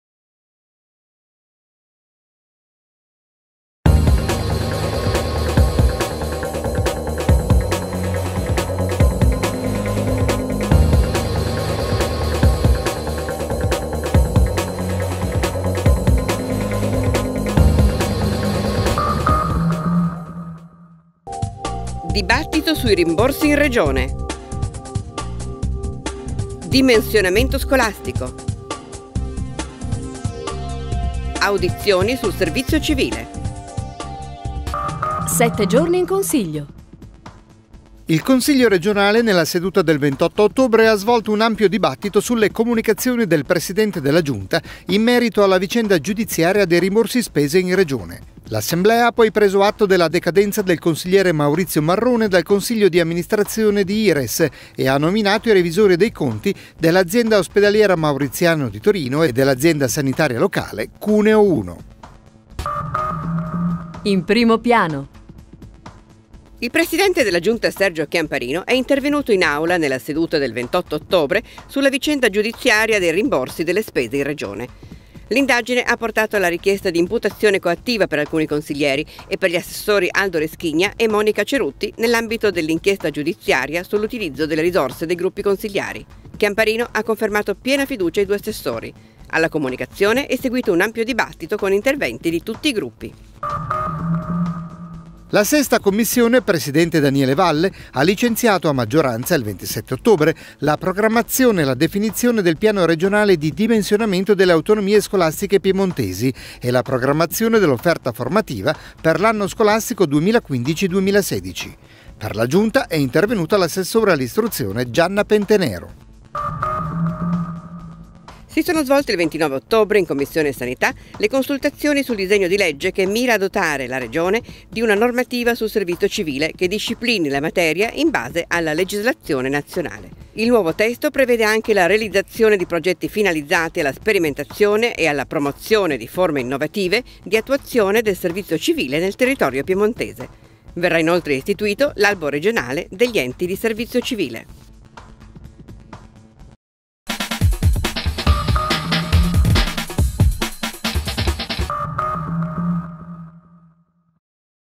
CrpTv n.26 del 31 Ottobre 2014
Dibattito sui rimborsi in Regione
Dimensionamento scolastico
Audizioni sul servizio civile

Il Consiglio regionale, nella seduta del 28 ottobre, ha svolto un ampio dibattito sulle comunicazioni del presidente della Giunta in merito alla vicenda giudiziaria dei rimborsi spese in Regione. L’Assemblea ha poi preso atto della decadenza del consigliere Maurizio Marrone dal Consiglio di Amministrazione di Ires e ha nominato i revisori dei conti dell’Azienda ospedaliera Mauriziano di Torino e dell’Azienda sanitaria locale Cuneo 1.

PRIMO PIANO

01) Dibattito sui rimborsi in Regione
Il presidente della Giunta Sergio Chiamparino è intervenuto in Aula, nella seduta del 28 ottobre, sulla vicenda giudiziaria dei rimborsi delle spese in Regione. L'indagine ha portato alla richiesta di imputazione coattiva per alcuni consiglieri e per gli assessori Aldo Reschigna e Monica Cerutti, nell’ambito dell’inchiesta giudiziaria sull’utilizzo delle risorse dei gruppi consiliari. Chiamparino ha confermato piena fiducia ai due assessori. Alla comunicazione è seguito un ampio dibattito con interventi di tutti i gruppi.

02) Dimensionamento scolastico
La sesta Commissione, presidente Daniele Valle, ha licenziato a maggioranza il 27 ottobre, la programmazione e la definizione del piano regionale di dimensionamento delle Autonomie scolastiche piemontesi e la programmazione dell’offerta formativa per l’anno scolastico 2015/2016. Per la Giunta è intervenuto l'assessore all'Istruzione Gianna Pentenero.

03) Servizio civile
Si sono svolte il 29 ottobre, in Commissione Sanità, le consultazioni sul disegno di legge che mira a dotare la Regione di una normativa sul servizio civile, che disciplini la materia in base alla legislazione nazionale. Il nuovo testo prevede anche la realizzazione di progetti finalizzati alla sperimentazione e alla promozione di forme innovative di attuazione del servizio civile nel territorio piemontese. Verrà inoltre istituito l'albo regionale degli enti di servizio civile.